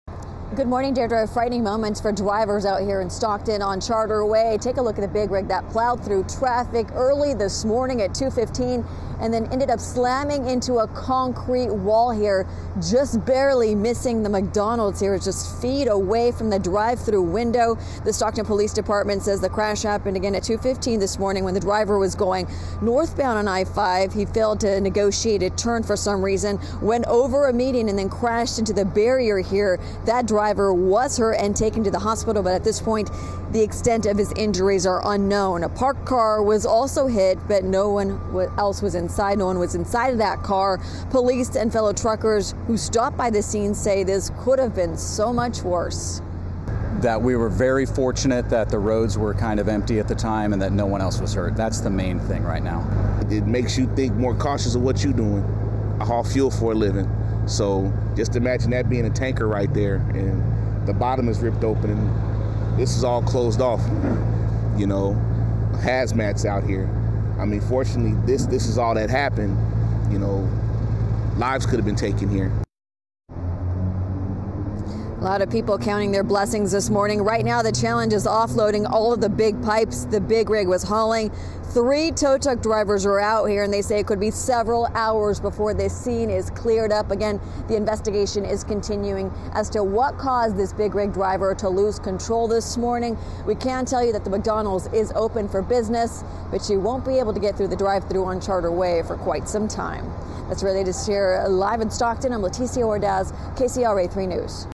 Big rig crashes into Stockton McDonald's parking lot
Crash happened around 1:45 a.m. off Charter Way.